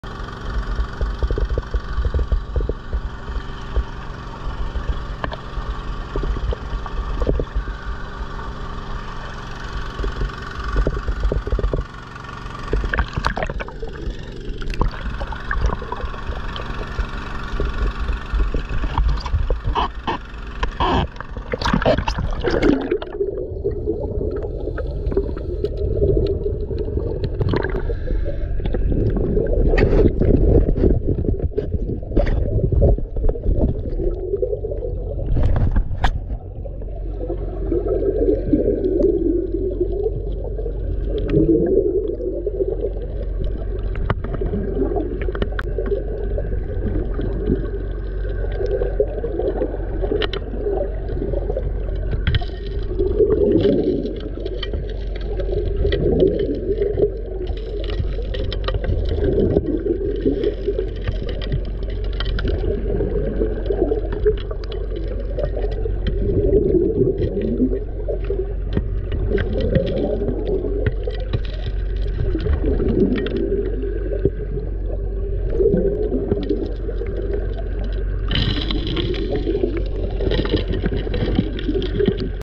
4” dredge time!
Starting a new hole with my 4 inch stretch